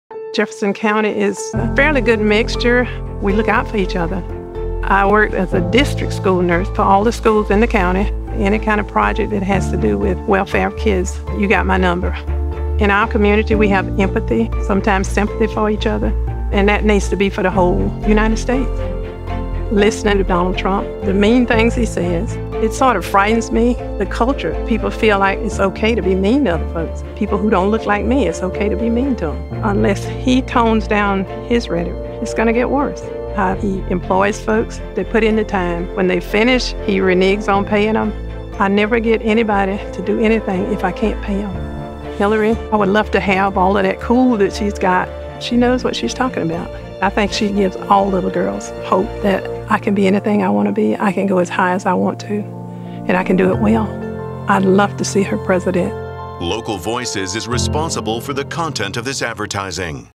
Meet Gladys: A former District School Nurse from Jefferson County, Florida
Gladys has spent her adult life caring for children in her community, and she believes empathy is a quality we need more of in this country.